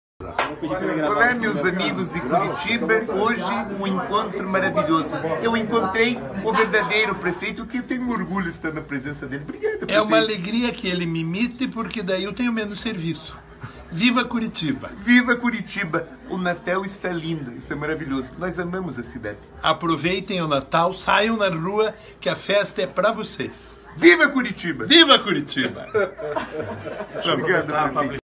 Prefeito Rafael Greca e seu imitador (😅😅😅)
Ria se puder - Prefeito Rafael Greca ao lado de seu Imitador (kkk)